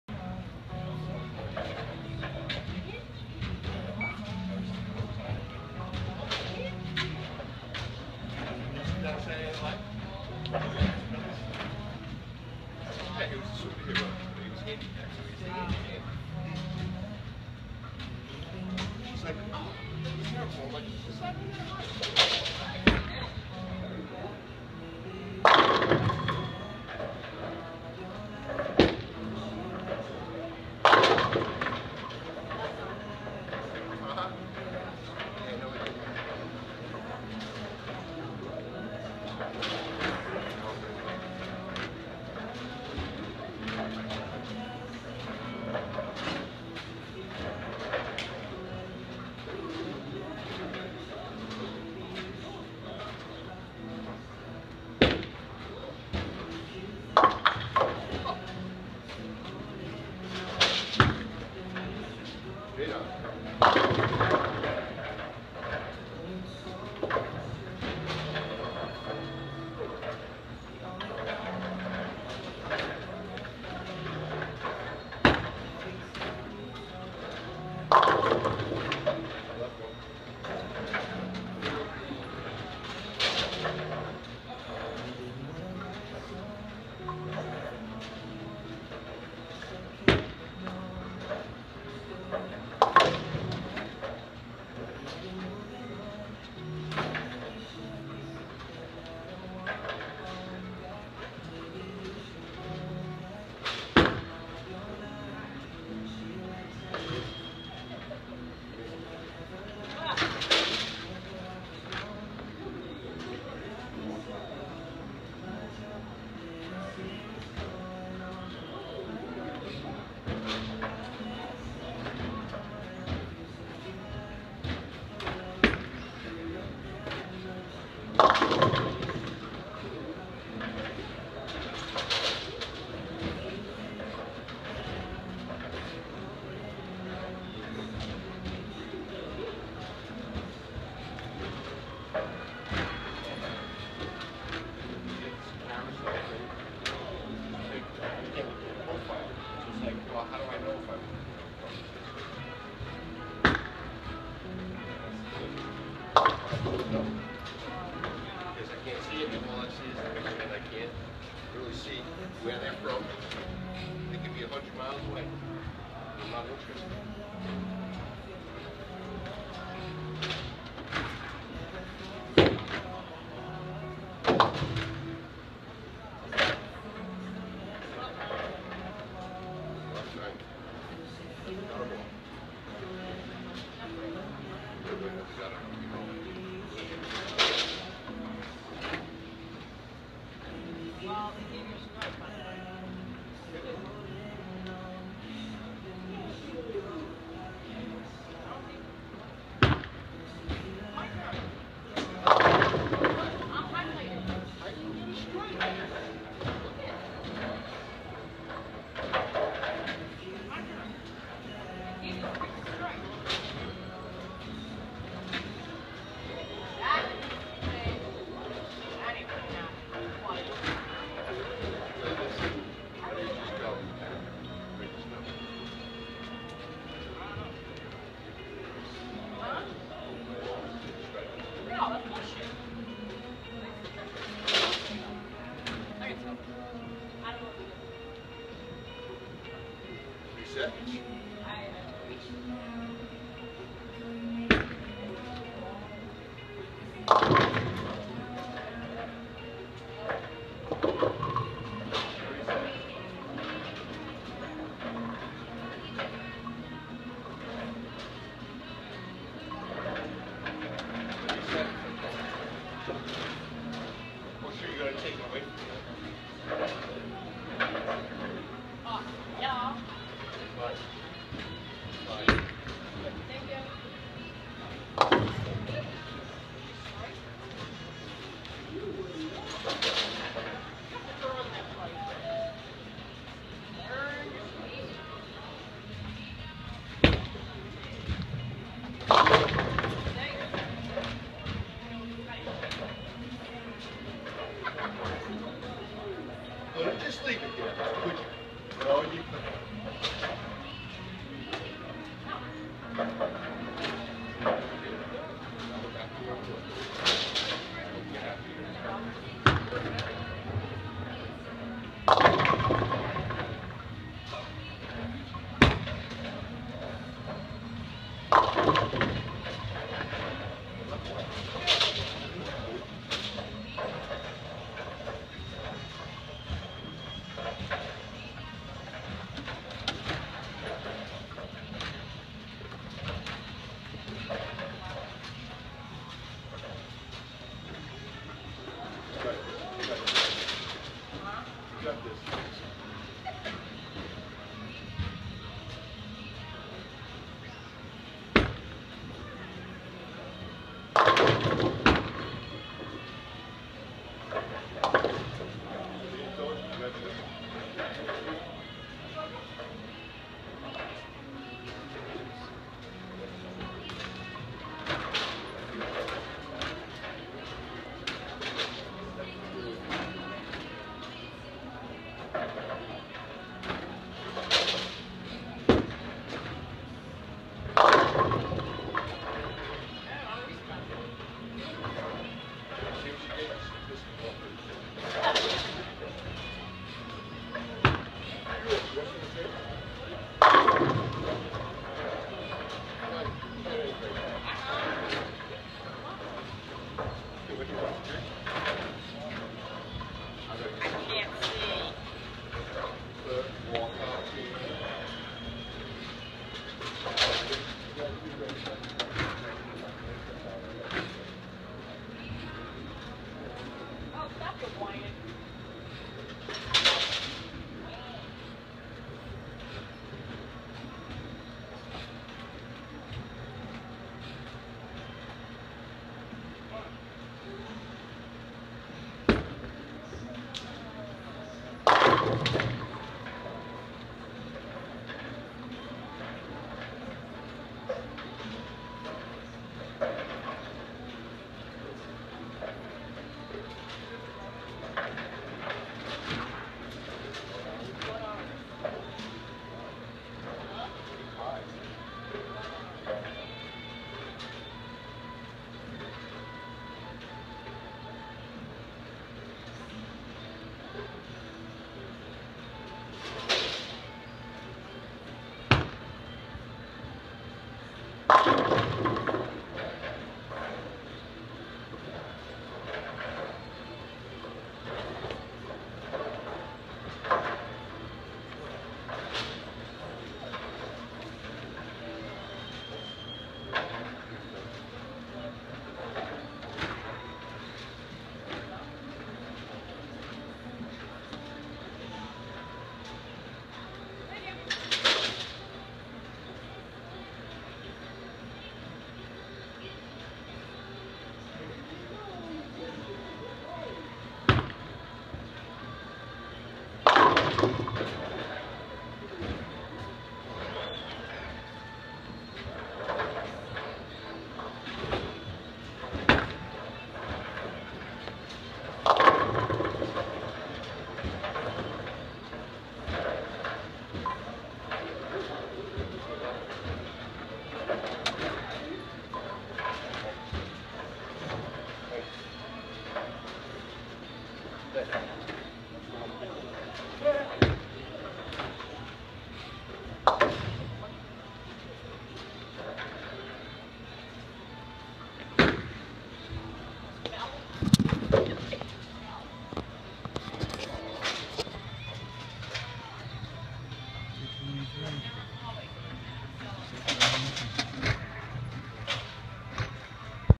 Me bowling 223 at east providence
Me bowling solid game bowling 223 at east providence. I'm bowling with my storm crux.